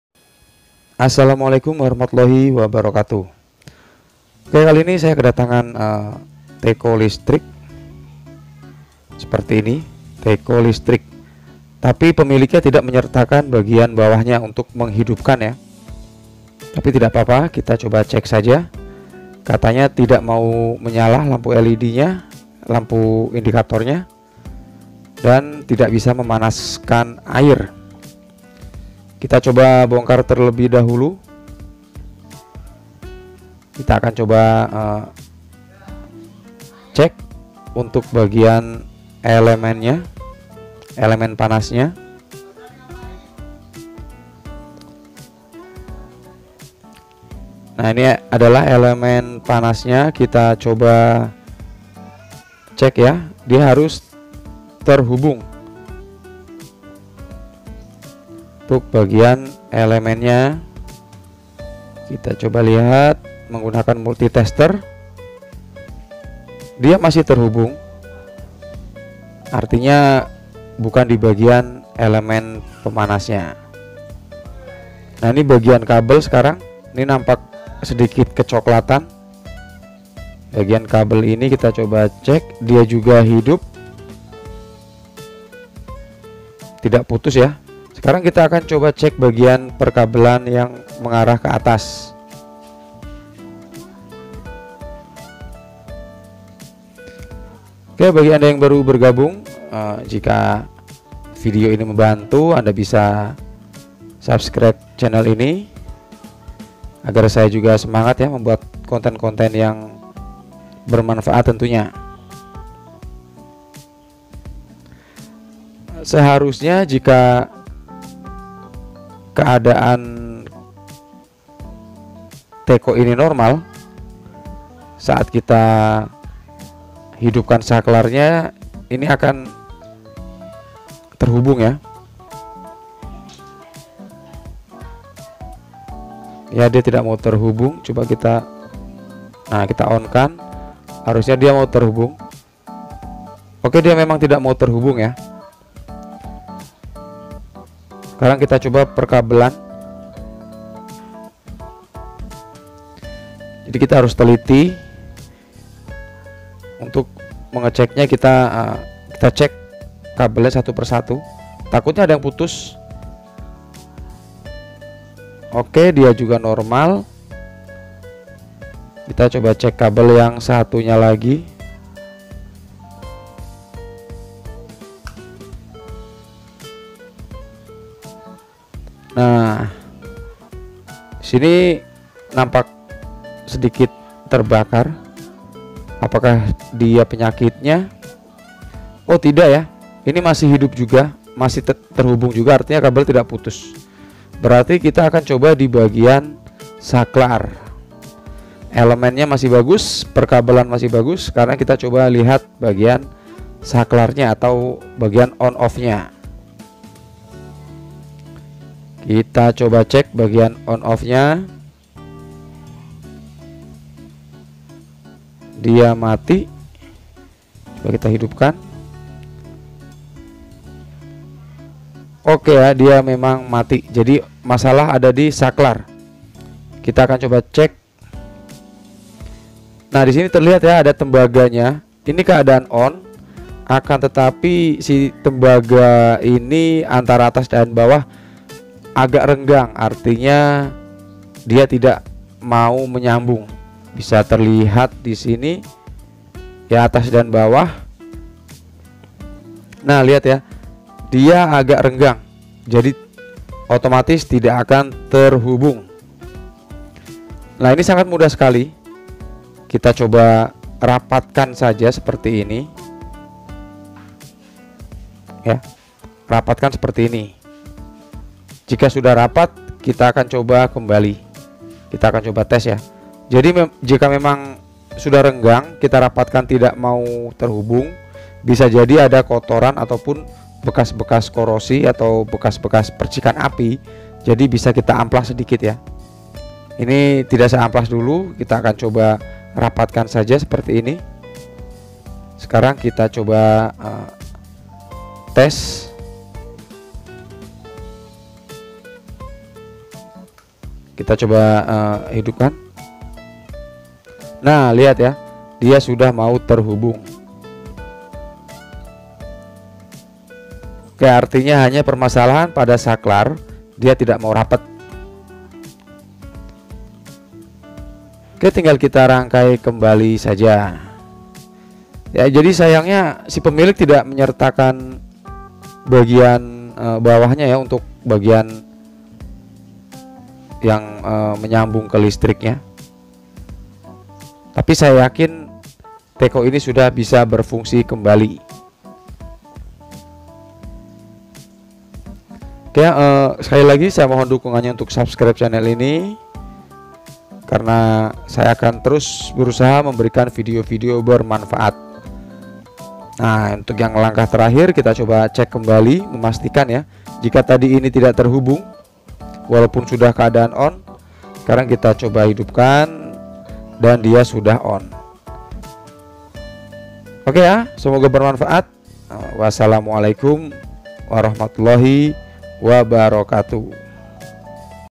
Service Teko Listrik Tidak Menyala
teko listrik tidak menyala lampu indikatornya dan air tidak bisa mendidih. Begini caranya